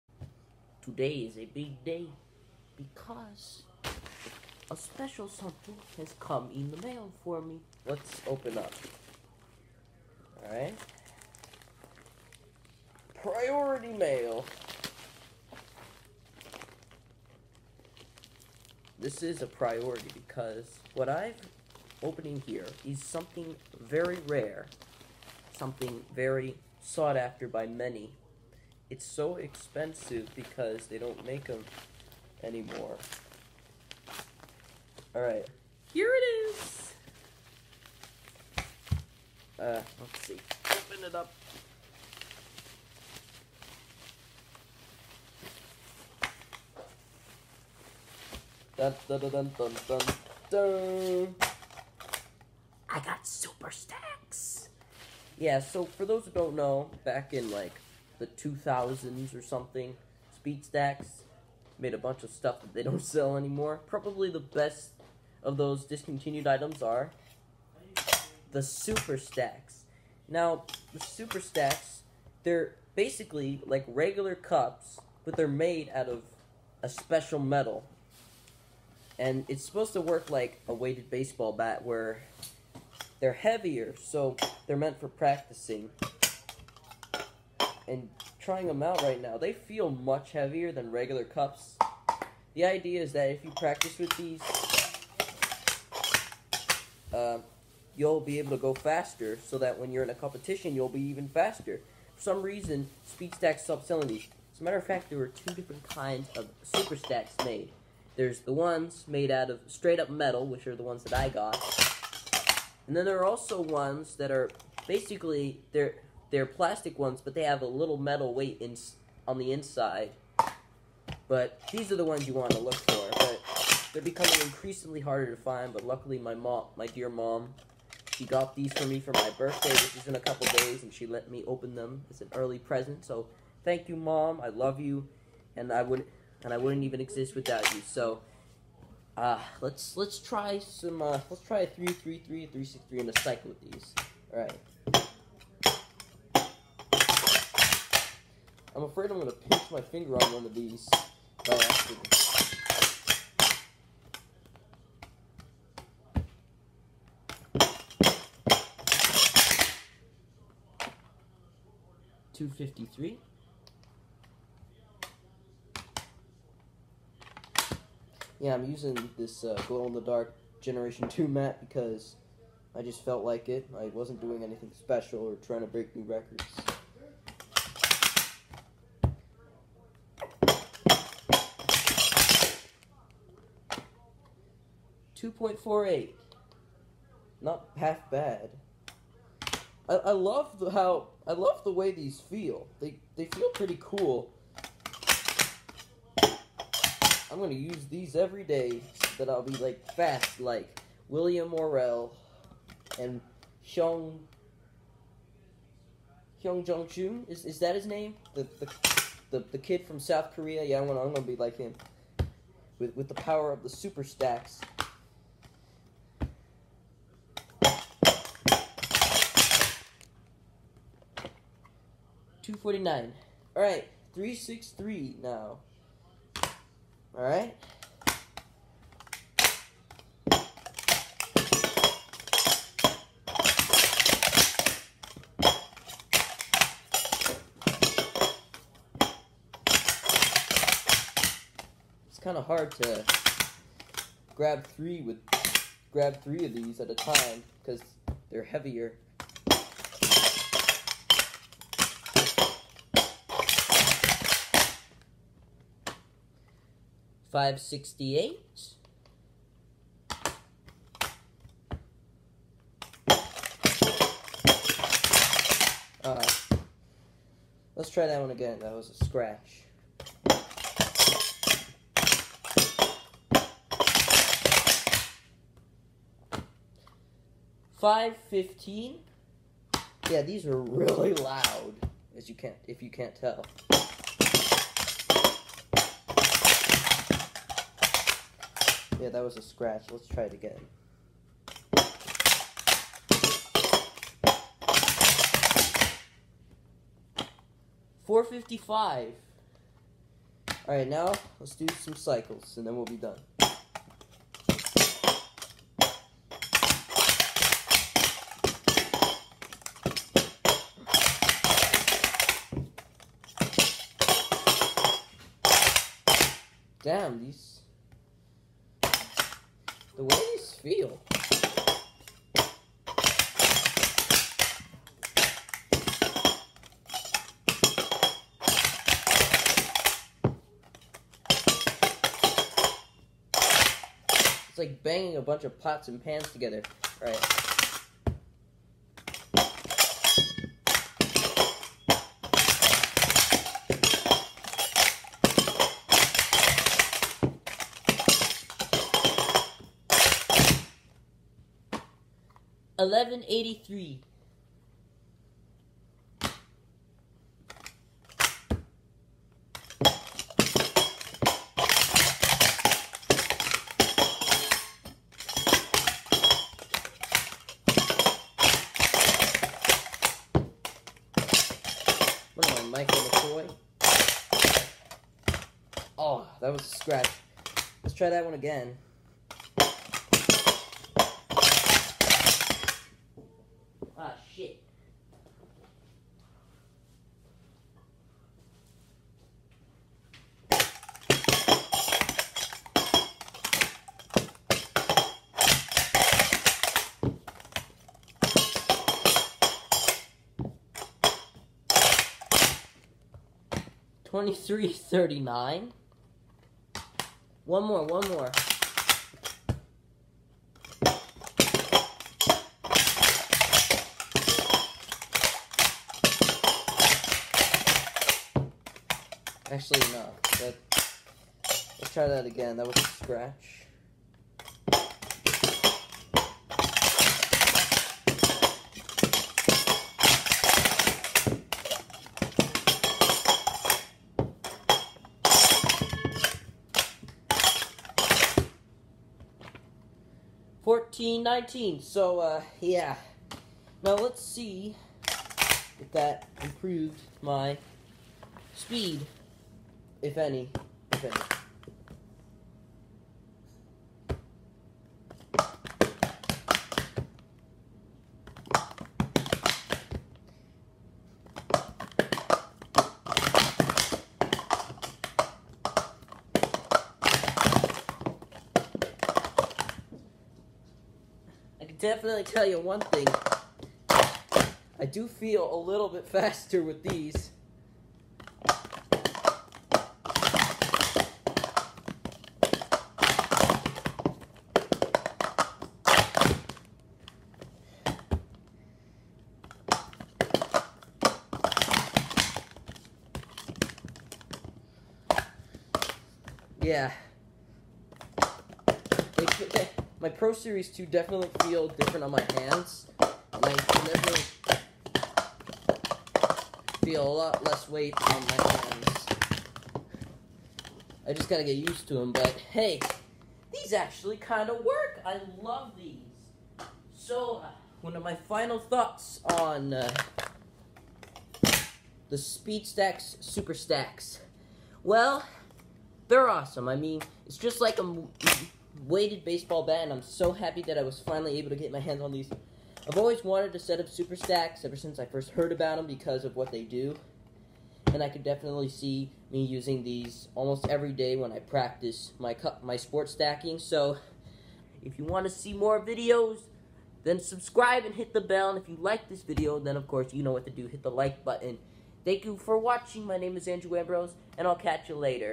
Super Stacks Unboxing
Recorded on Apr 13, 2019
My 15th birthday is this Friday, and my mom let me open one of my presents early...